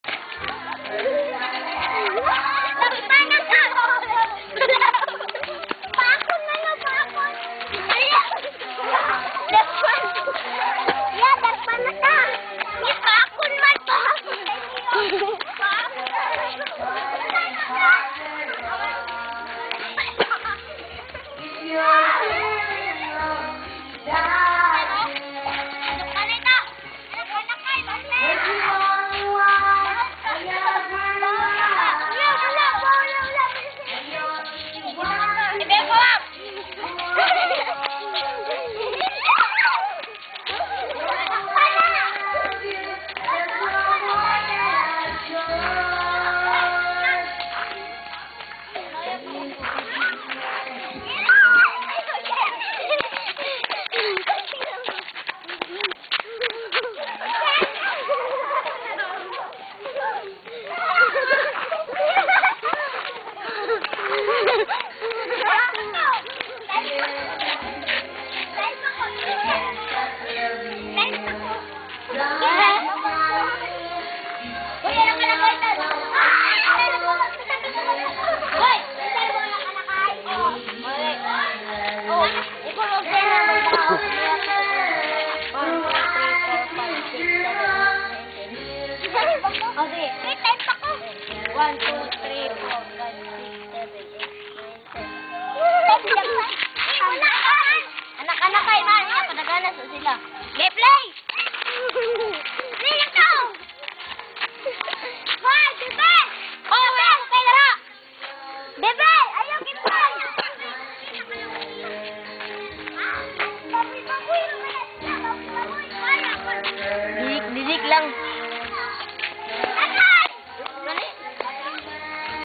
during my birthday afternoon
my niece's and nephew's were playing...